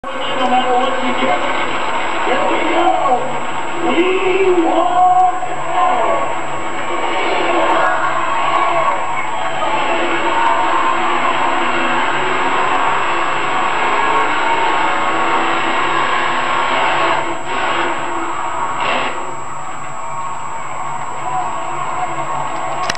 California Kid Trailer Jump. Eureka, Cal Monster Trucks 1997
California Kid Hillbilly Trailer Jump. From Eureka, California. Mike Welch Monster Trucks 1997